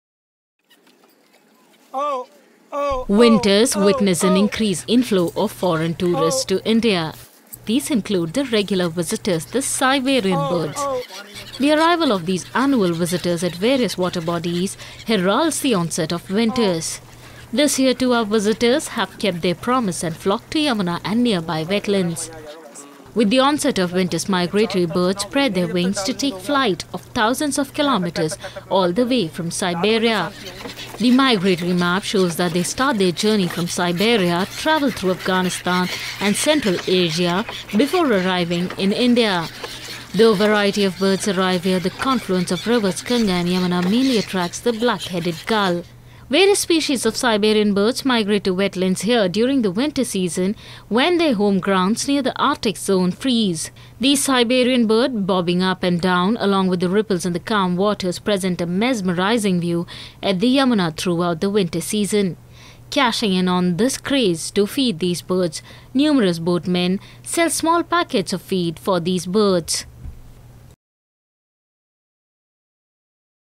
Migratory birds keep their winter date with Delhi
Like every year, this year too migratory birds have kept their promise and flocked to Yamuna and nearby wetlands. With the onset of winters, migratory birds spread their wings to take flight of thousands of kilometres all the way from Siberia to India.